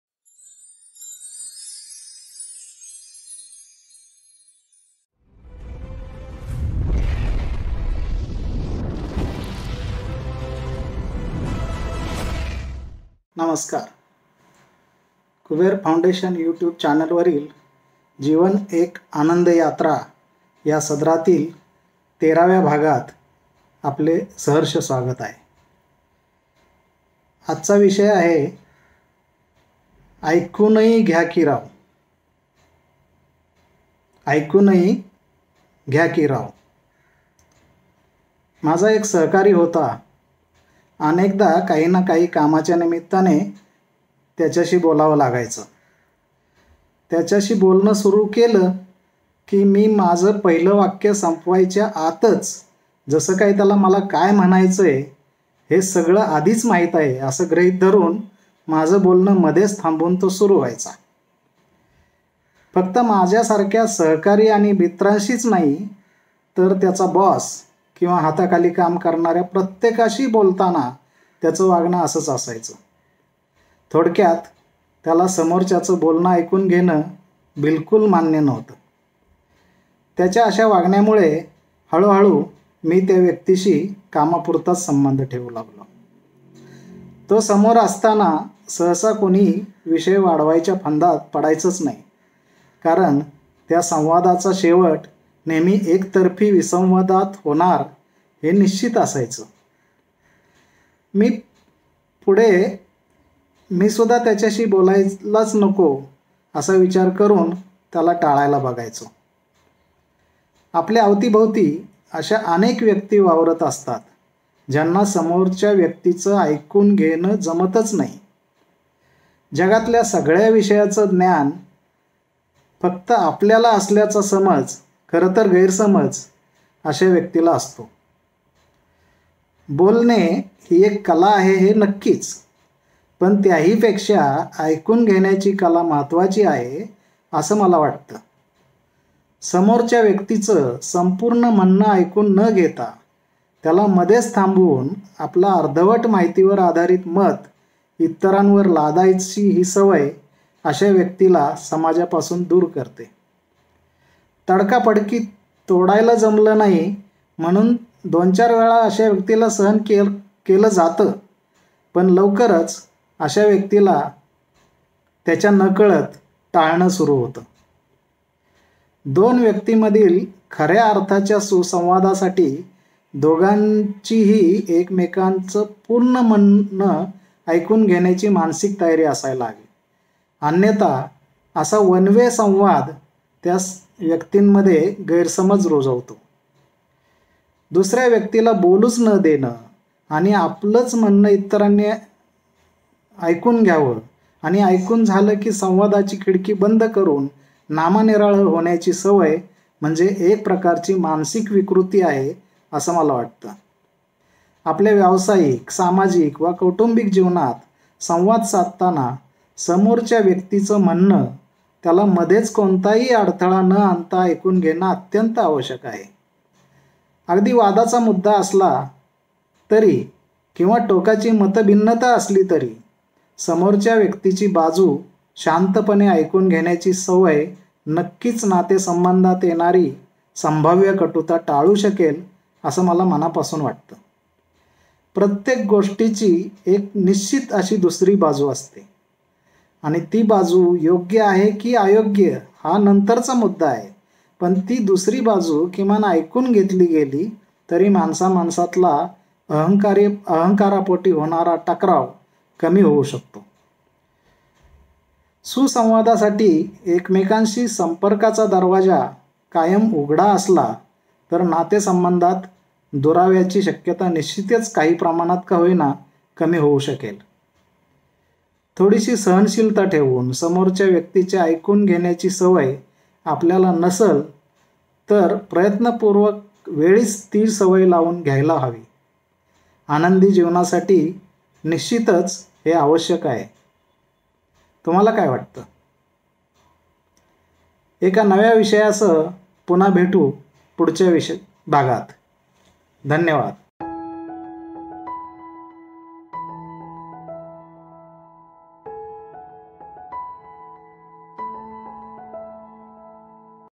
ऐकूनही घ्या की राव | मना दर्पणा भाग 12 | प्रल्हाद दुधाळ
'जीवन एक आनंदयात्रा' हे सदर चालू होते आहे. दर बुधवारी आपल्या 'मना दर्पणा' या पुस्तकातील एक भाग श्री प्रल्हाद दुधाळ आपल्या विडिओ मध्ये अभिवाचन करतील.
आजचा भाग 'ऐकूनही घ्या' .
नक्की पहा.

बोलताना काही लोक फक्त स्वतःच बोलत रहातात,मात्र दुसऱ्याची बाजू  ऐकून घेणे त्यांना जमत नाही  दोन माणसातील सुसंवादासाठी एकमेकांशी बोलण्याबरोबरच एकमेकांचे ऐकूनही घेतले जाणे आवश्यक आहे. अशा वृतीबाबतचा हा लेख ...
   'ऐकूनही घ्या की राव!'

माणूस  शक्यतो आपल्या मनातले विचार कुणाशीही शेअर न करता स्वतःच  विचारात हरवलेला असतो. अतिविचार केल्याने तो मनोकायिक आजारांना बळी पडू शकतो म्हणूनच जेव्हा वैचारिक गोंधळ असेल तेव्हा योग्य त्या व्यक्तीशी मनातलं बोलून,सल्लामसलत करून निर्णय घेतले तर असे विचारांचे गुंते सोडवता येतील .
मनातल्या विचारांबद्दलचा हा लेख 'विचारांचे गुंते'...